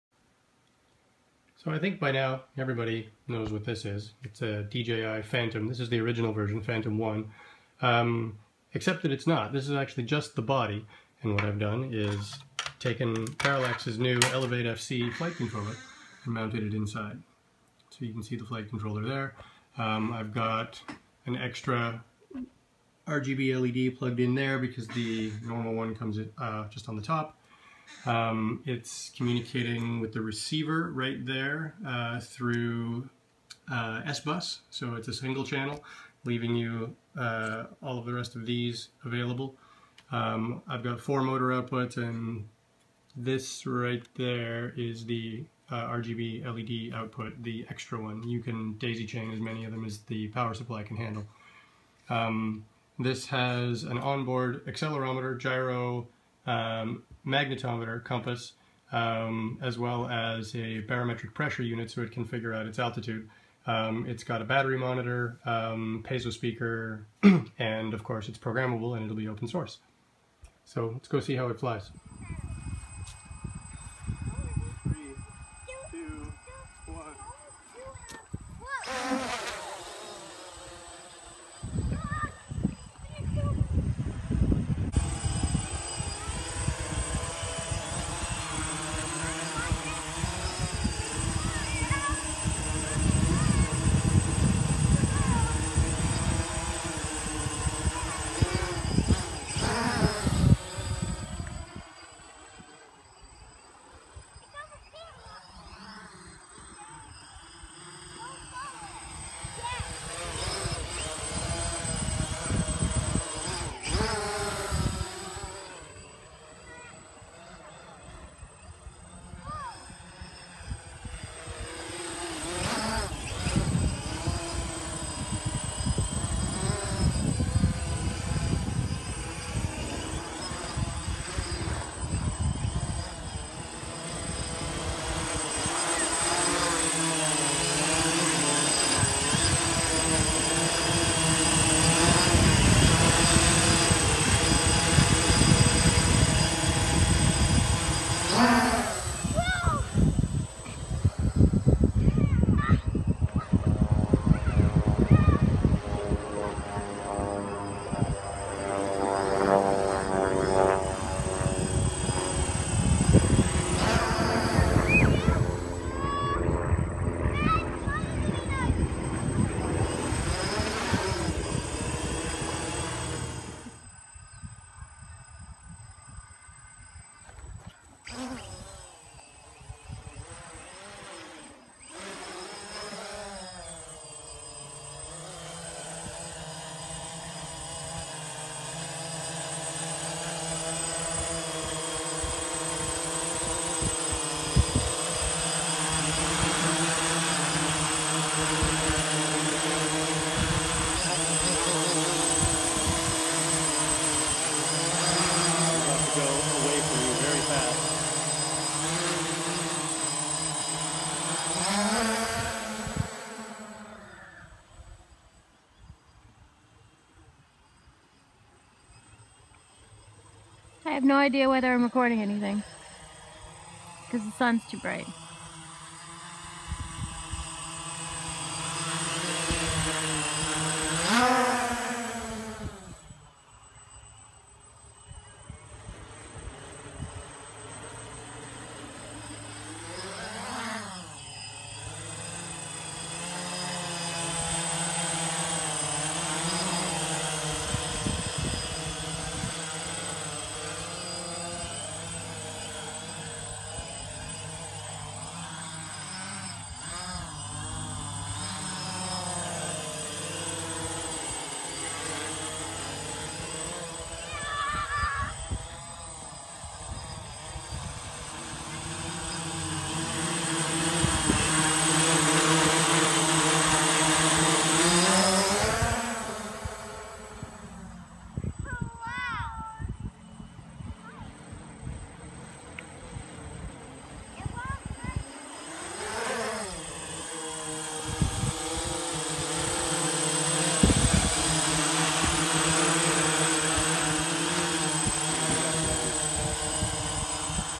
Elev8-FC flight demo
A quick look at Parallax's new Elev8-FC flight controller.

Hardware includes a Parallax Propeller, an LSM9DS1 gyro/accelerometer/magnetometer, a WS2812B status LED, LPS22H barometric pressure sensor, 8 remote inputs, 6 ESC outputs, I2C I/O header, WS2812B output (for chaining LEDs), an expansion port, and a piezo speaker.

Includes open source code to drive it all, with resources left over for user code and experimenting.

I bought a body shell for a Phantom 1, mounted the Elev8-FC to it, and it flies like a champ.  If you don't believe me, you can tell it's not a Phantom by the blink pattern of the LED.  :-)